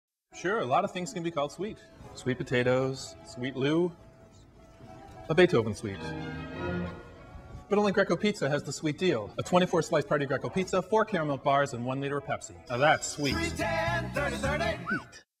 Greco Pizza - "Sweet Deal - Sweet Lou"
A man walks through an exhibit of things that are sweet. Nothing compares to how 'sweet' the Greco Sweet Deal is.